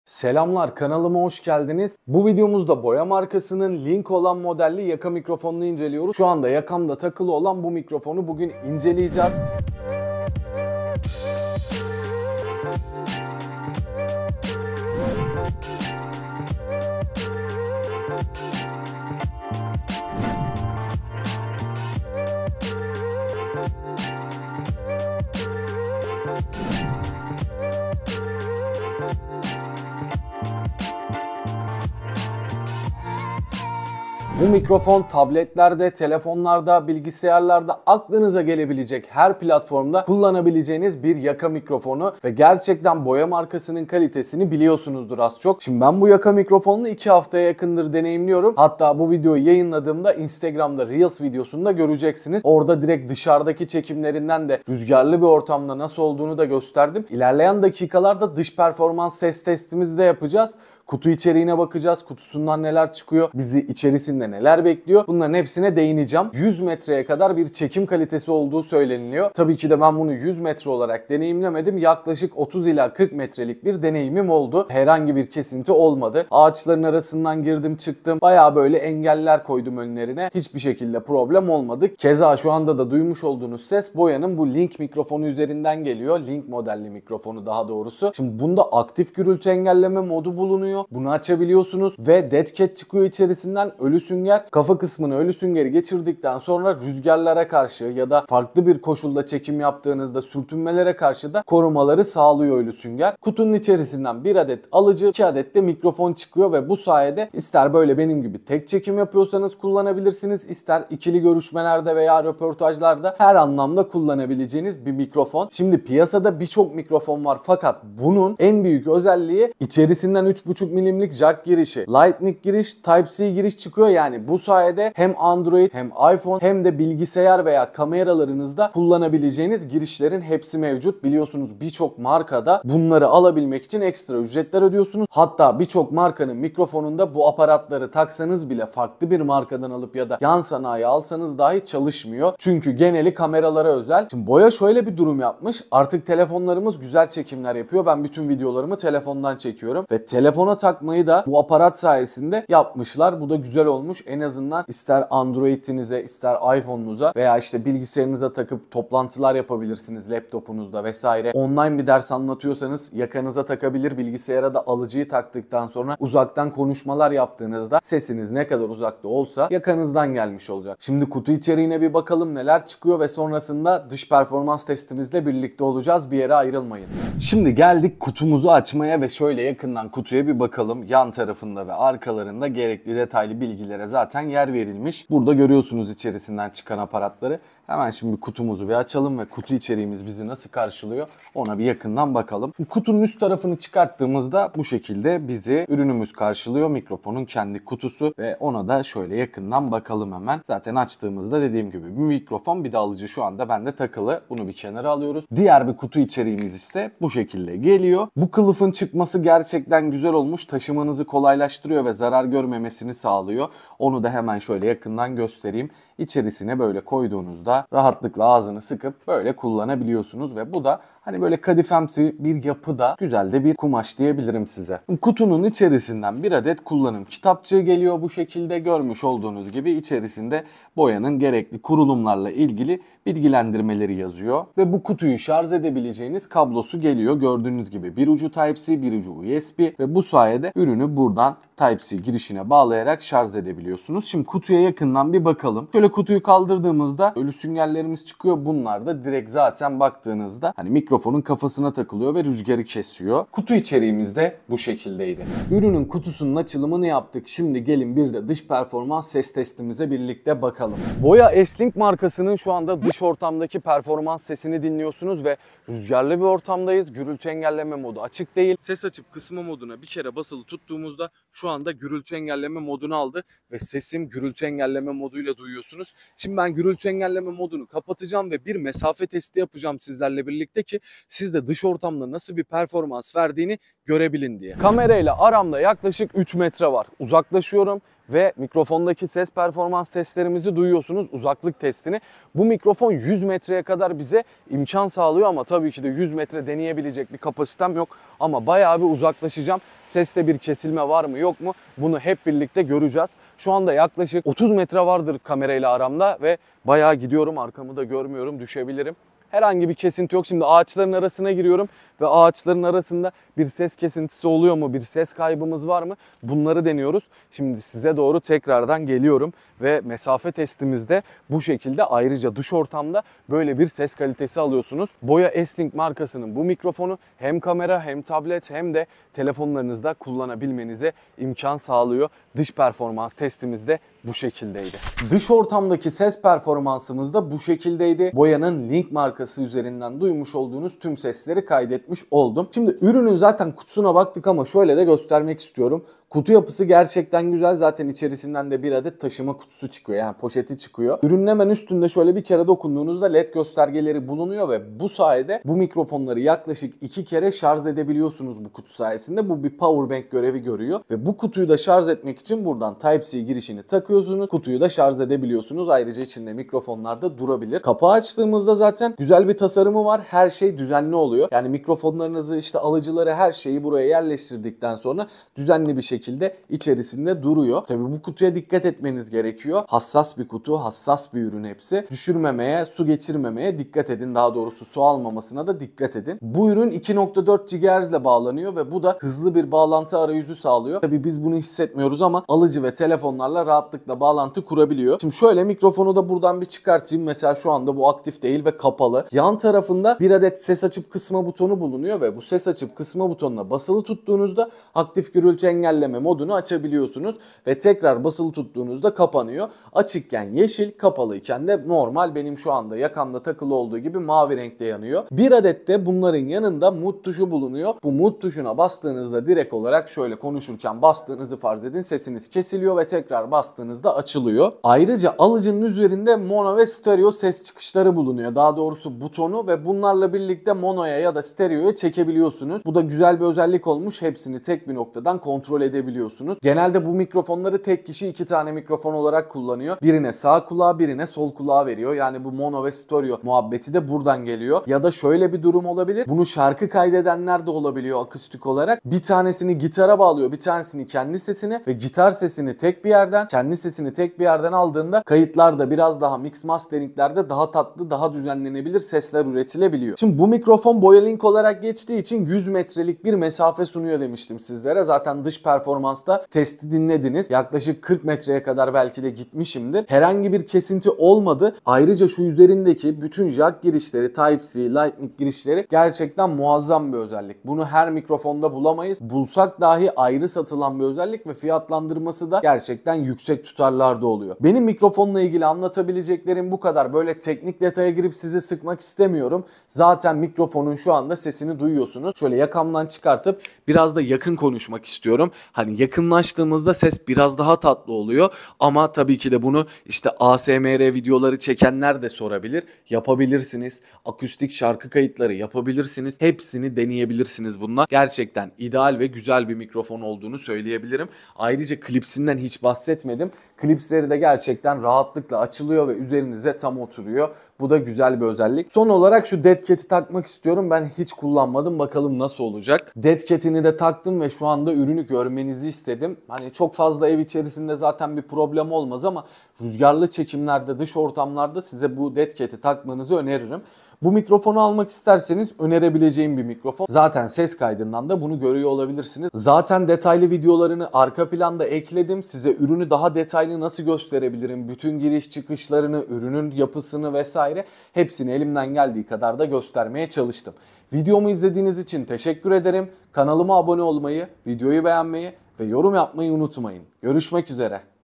Android & İphone & Tablet & Bilgisayar Uyumlu Kablosuz Mikrofon BOYA LİNK
Boya Link modelli kablosuz yaka mikrofon incelemesini yaptım mikrofonun iç ve dış ses performansını test ettik ve kutu içinden çıkanlara baktım iyi seyirler